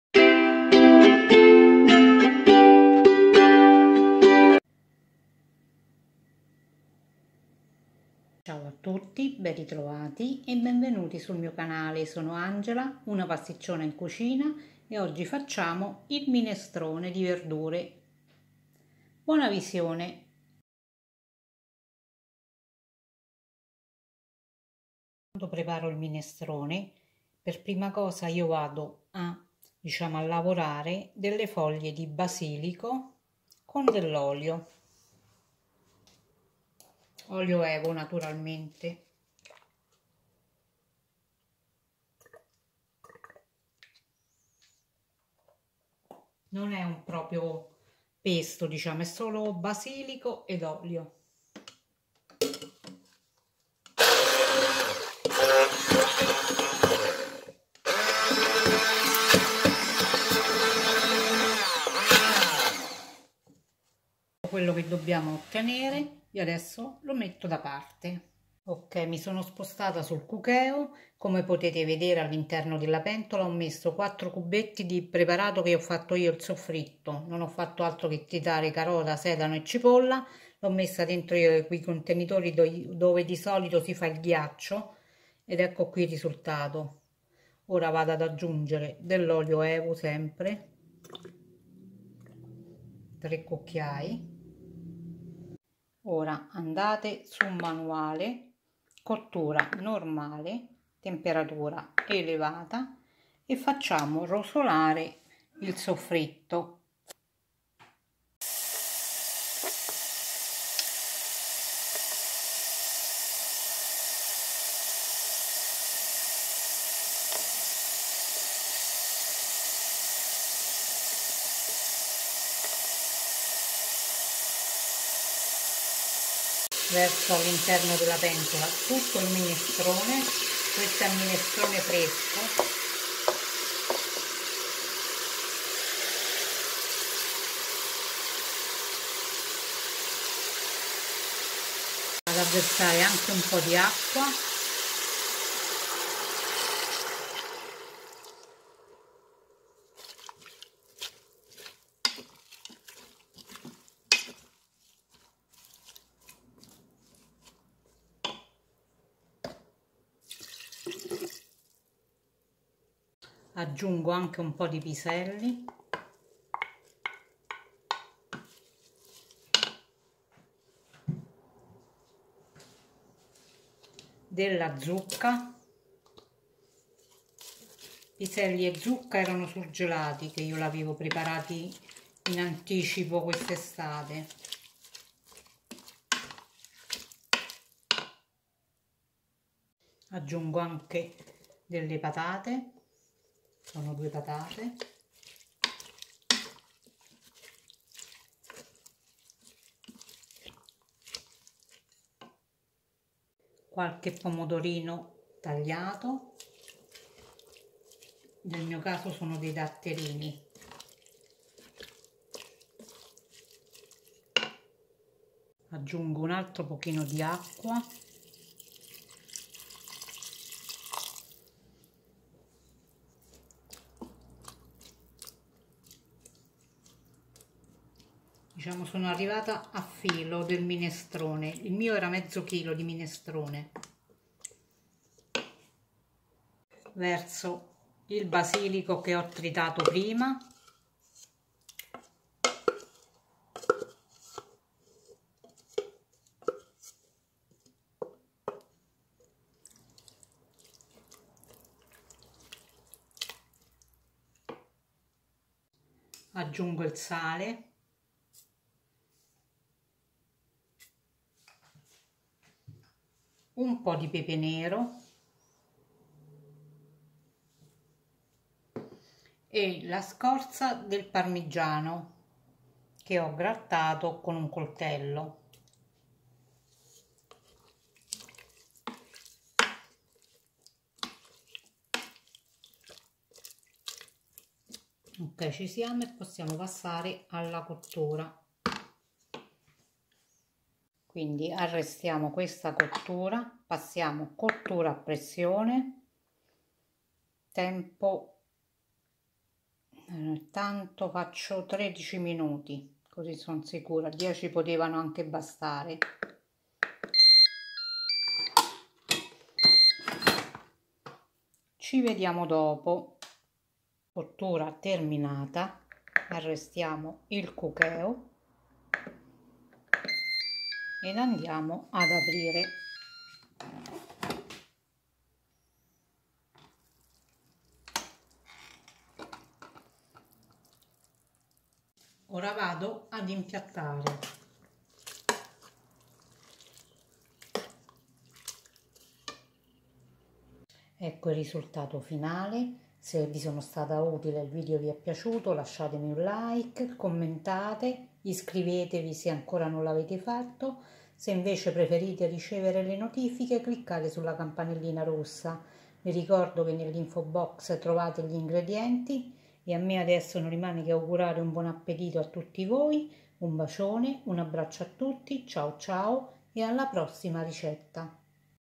___ minestrone __ ❤ cookeo ❤
Minestrone dal sapore delicato, come sempre facile  e veloce. Cookeo non delude mai. Puoi prepararlo anche con la pentola a pressione elettrica che hai tu.

INGREDIENTI:

Per la salsa al basilico:
foglie di basilico,
olio e.v.o.

Per il minestrone:
soffritto (carota, sedano  cipolla),
3 cucchiai d'olio e.v.o.,
500 g di minestrone,
1300 ml di acqua
150 g di piselli (io surgelati),
200 g di zucca ( io surgelata),
2 patate medie a tocchetti,
7 pomodorini datterini  a pezzetti,
2 cucchiaini di sale,
pepe nero,
la scorza del parmigiano (facoltativo).

COSA HO UTILIZZATO
frullatore ad immersione Braun